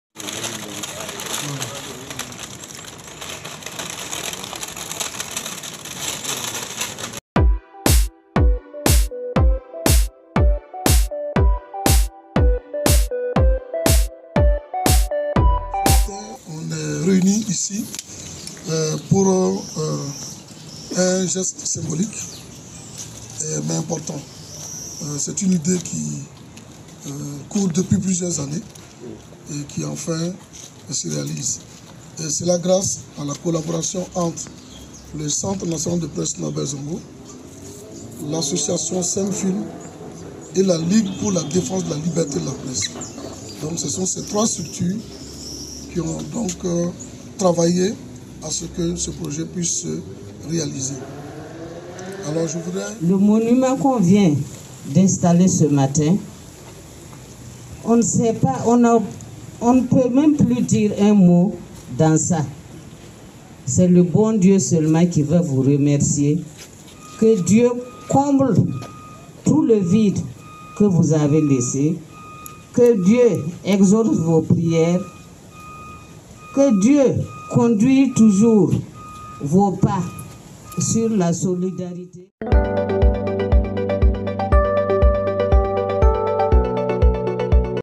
Norbert Zongo « accueille » désormais tout visiteur au centre national de presse
25 ans après l’assassinat du journaliste d’investigation Norbert Zongo, le Centre national de presse éponyme a inauguré un buste en son honneur ce mercredi 13 décembre 2023 à Ouagadougou. Cette œuvre sculpturale a été réalisée par l’artiste burkinabè Siriki Ky.